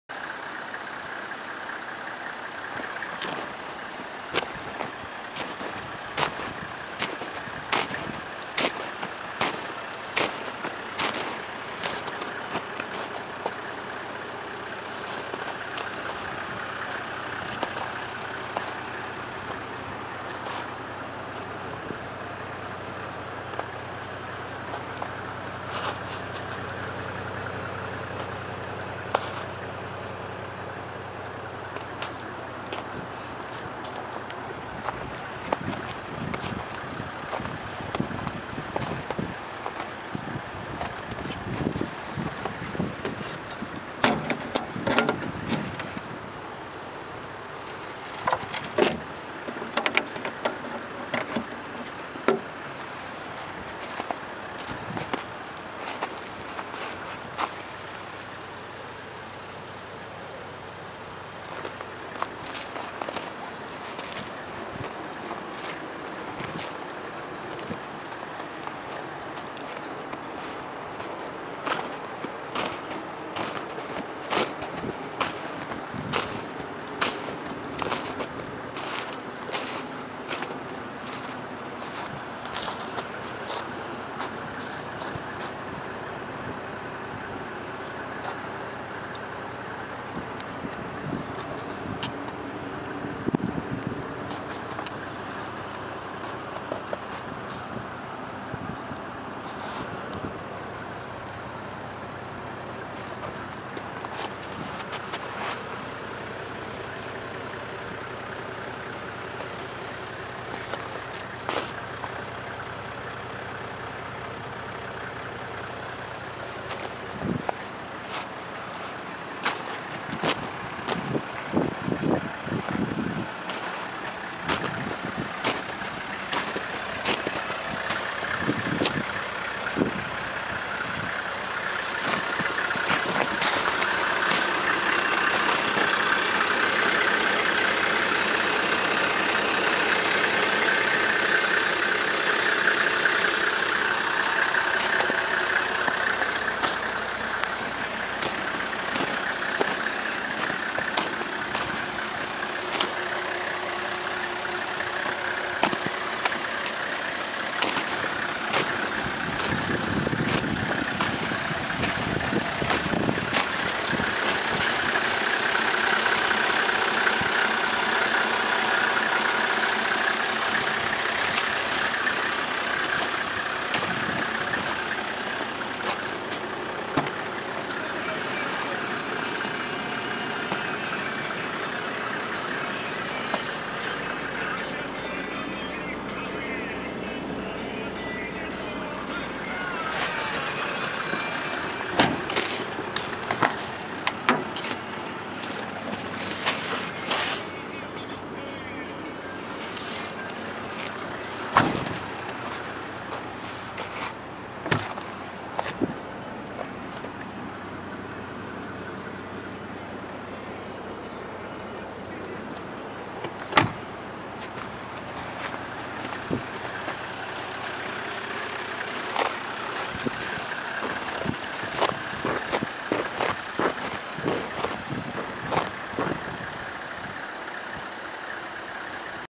Внедорожный прицеп Анвир (часть 1)
Прицеп сделан для автомобиля Тойота Ленд Крузер 70-й серии в компании АНВИР. Прицеп сделан на базе МЗСА и позволяет перевозить крупный квадрик или снегоход. Также произведены доработки по автомобилю.